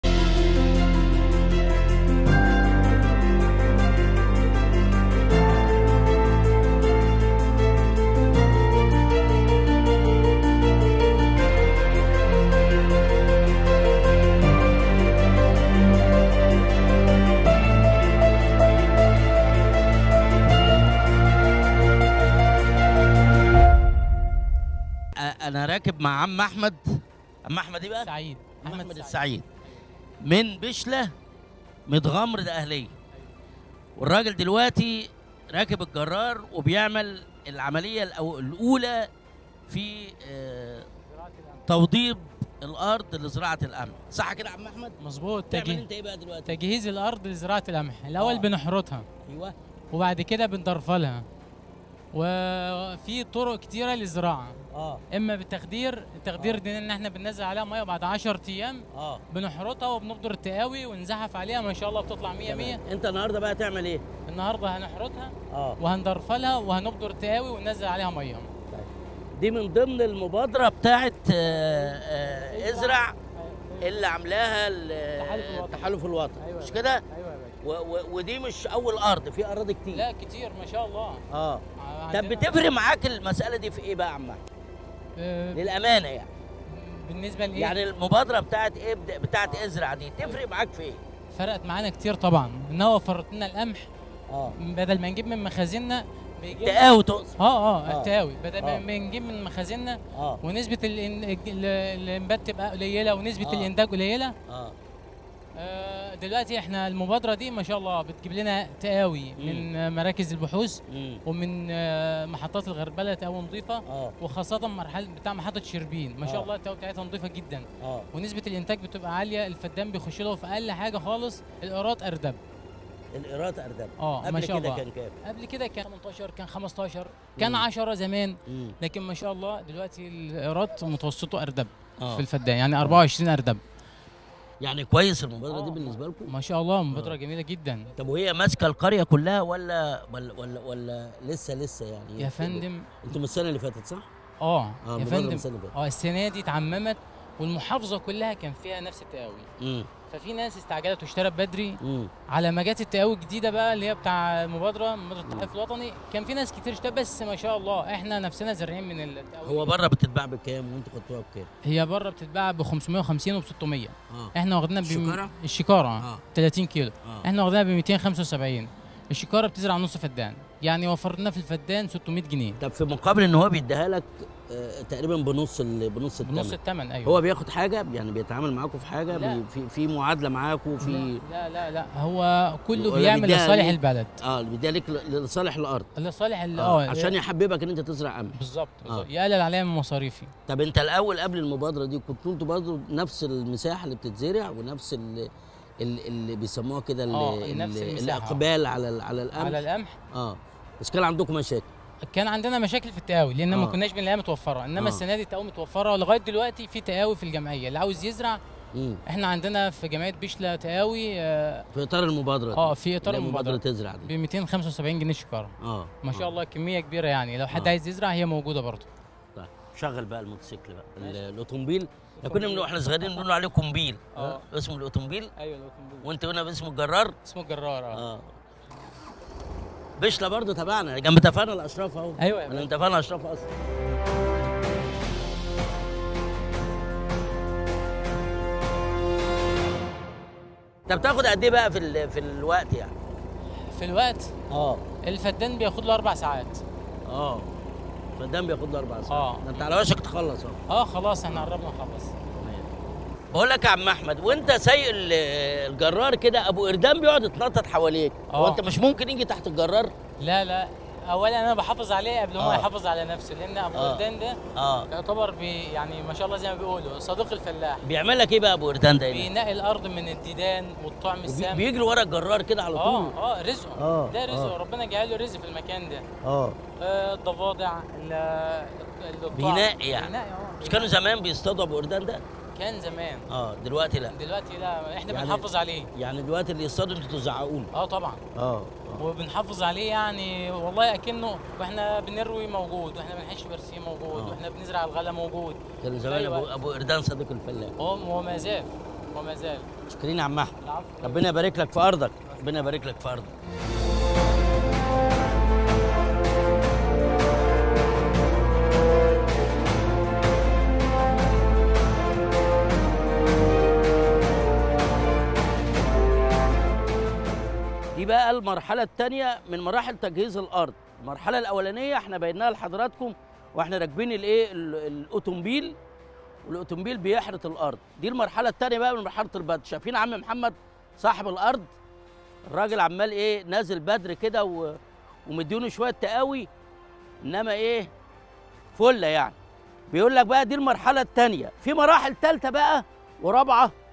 "بالجلابية والطاقية".. جابر القرموطي من داخل أرض زراعية تابعة لمبادرة "ازرع" بمحافظة الدقهلية
مانشيت| "بالجلابية والطاقية".. جابر القرموطي من داخل أرض زراعية تابعة لمبادرة "ازرع" بمحافظة الدقهلية، يحاور أحد الفلاحين